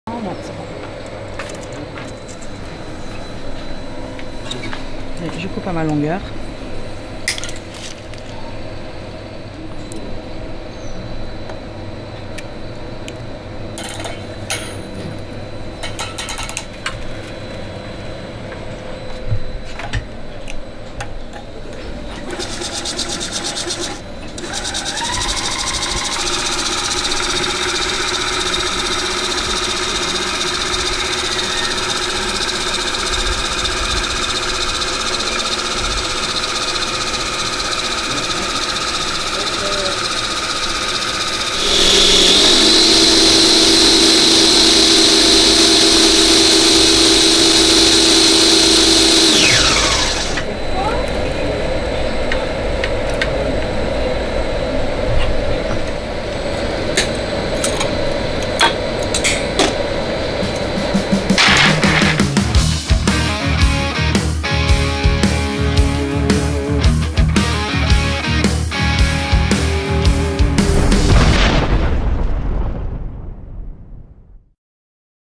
SAVAREZ | Fabrication d’une corde de basse
Bienvenue au coeur de l’usine Savarez, en plein dans la Drôme en France, où l’on fabrique nos cordes. Voici la réalisation d’une corde de basse électrique de la gamme Focus 🎸🎶

🎥Réalisation vidéo @matninatstudio